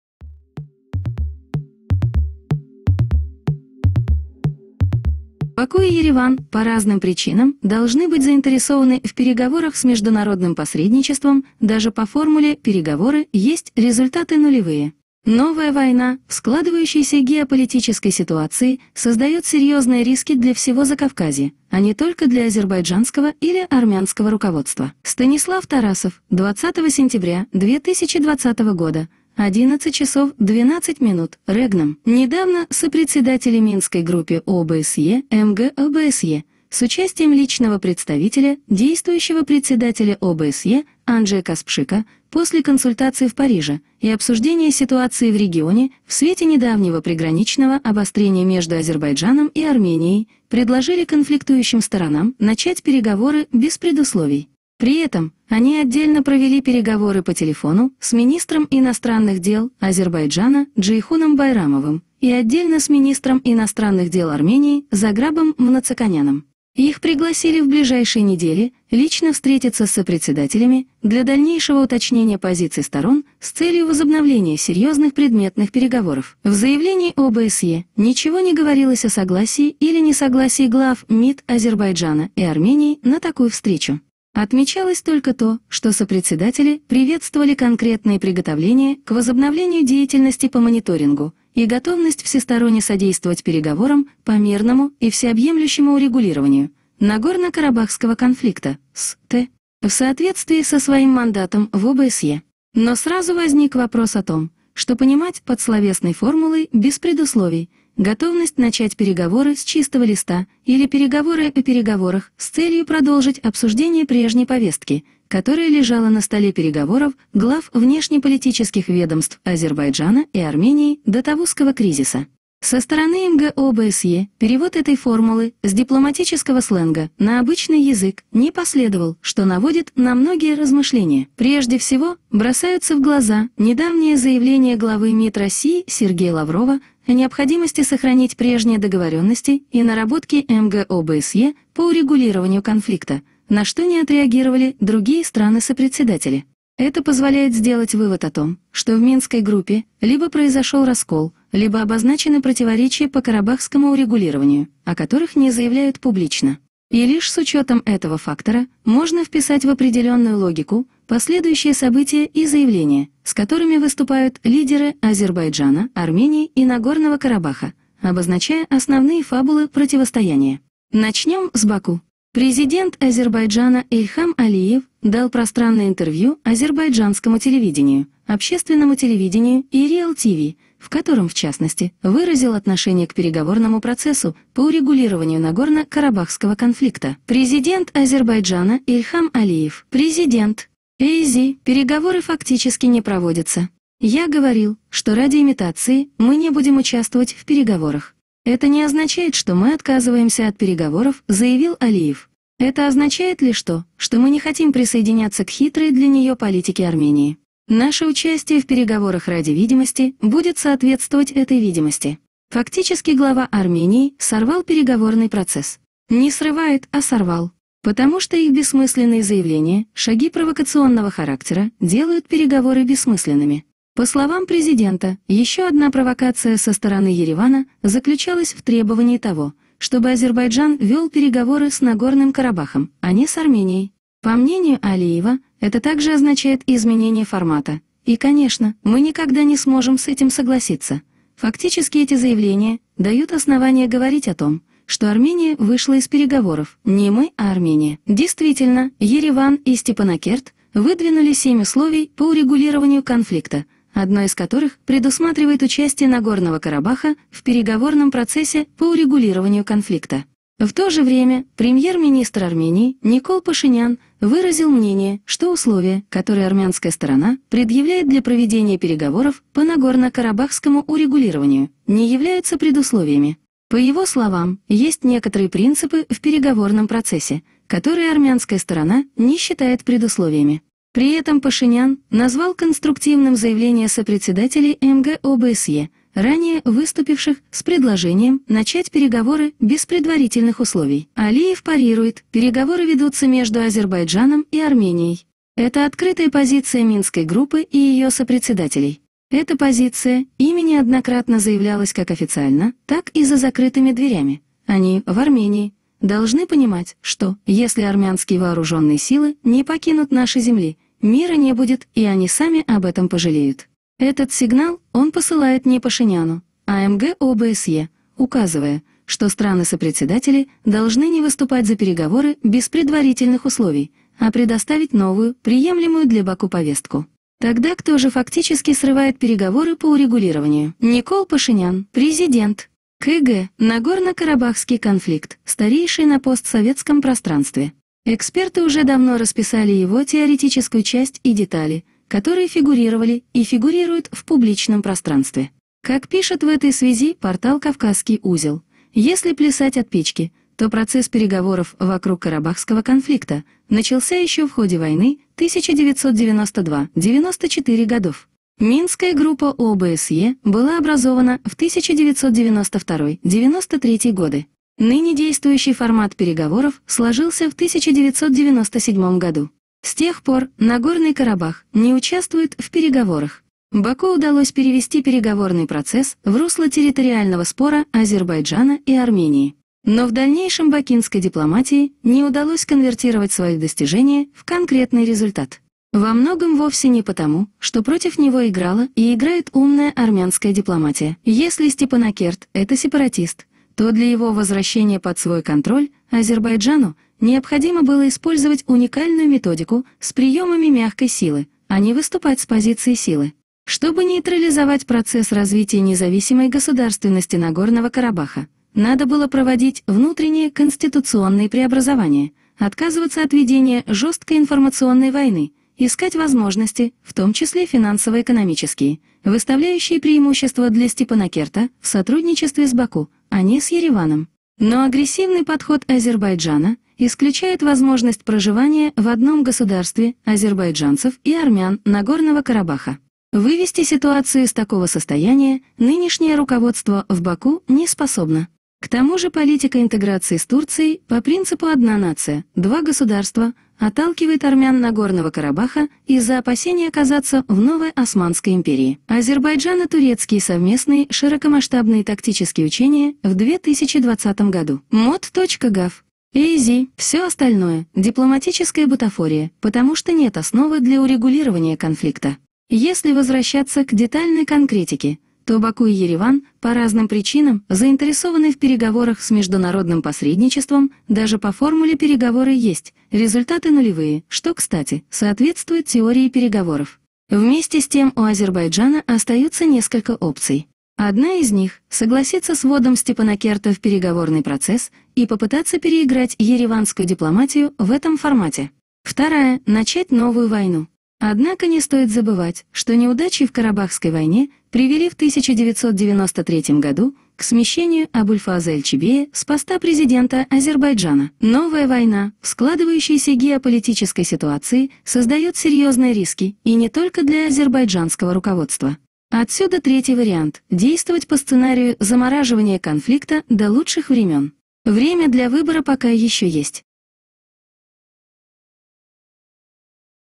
Кто Сорвал Важные Переговоры - Алиев Или Пашинян? [ Я ПРОСТО В ШОКЕ!!! ]
Недавно сопредседатели Минской группе ОБСЕ (МГ ОБСЕ) с участием личного представителя действующего председателя ОБСЕ Анджея Каспшика после консультаций в Париже и обсуждения ситуации в регионе в свете недавнего приграничного обострения между Азербайджаном и Арменией предложили конфликтующим сторонам «начать переговоры без предусловий». Армения и Азербайджан При этом они отдельно провели переговоры по телефону с министром иностранных дел Азербайджана Джейхуном Байрамовым и отдельно с министром иностранных дел Армении Зограбом Мнацаканяном. Армения и Азербайджан Их пригласили в ближайшие недели лично встретиться с сопредседателями для «дальнейшего уточнения позиций сторон с целью возобновления серьезных предметных переговоров»...

Любое использование материалов допускается только при наличии гиперссылки на ИА REGNUM.